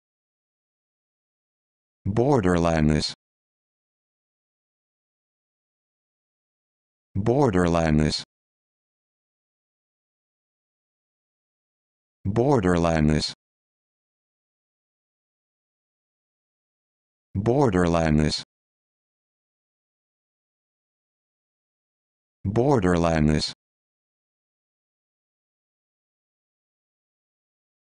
How to Pronounce Borderlineness
This video shows you how to pronounce Borderlineness